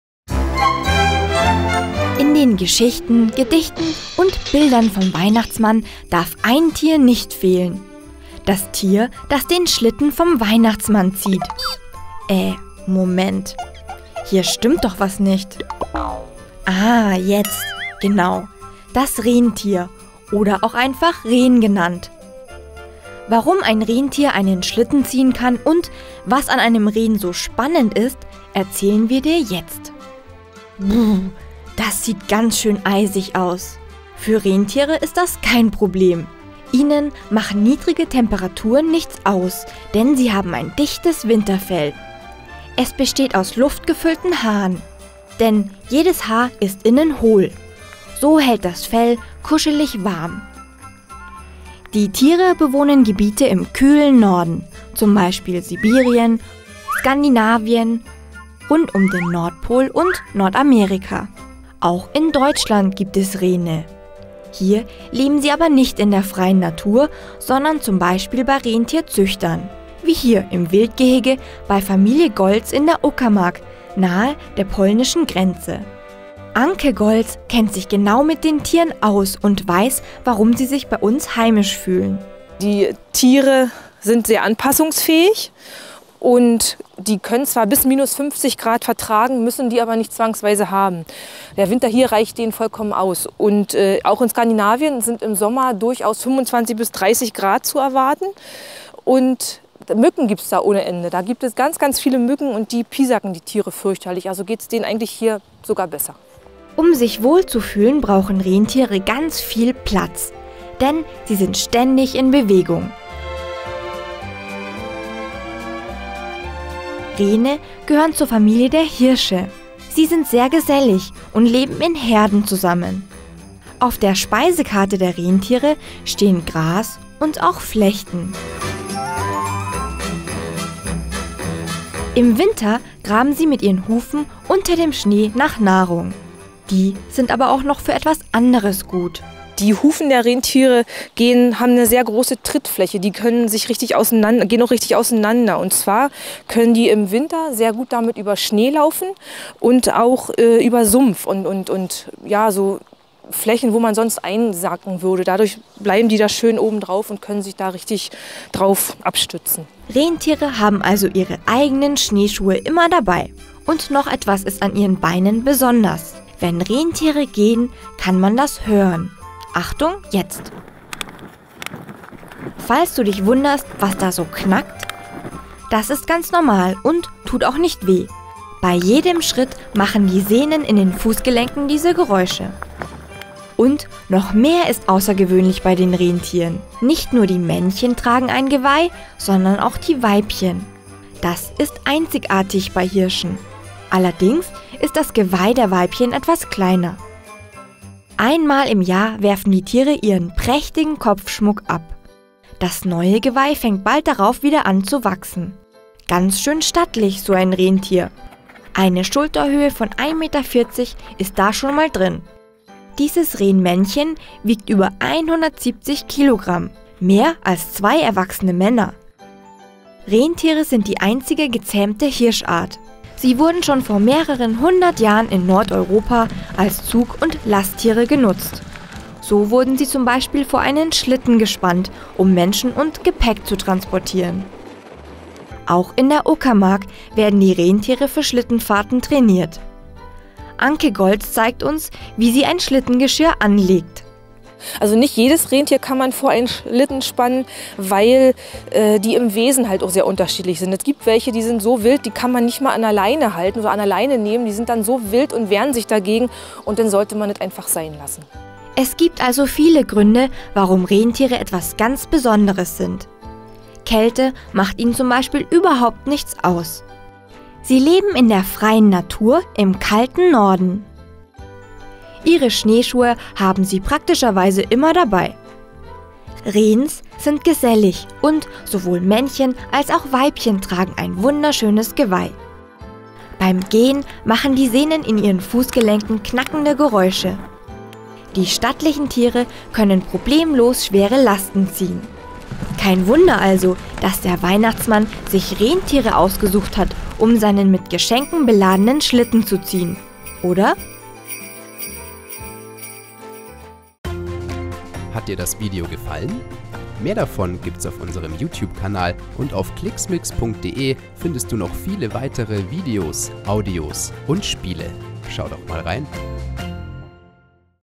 Wissensmix: Wie und wo leben Rentiere?
Rentiere können Schlitten ziehen, aber sie haben noch viel mehr auf dem Kasten!

Wusstest du, dass die Geschichte über die Rentiere des Weihnachtsmannes aus dem Jahr 1823 kommt? Wer das Gedicht verfasst hat, ist nicht mehr ganz klar. Sicher ist aber, dass das berühmte rotnasige Rentier Rudolph erst 1939 geboren wurde. Es stammt aus der Feder des Autos Robert L. May.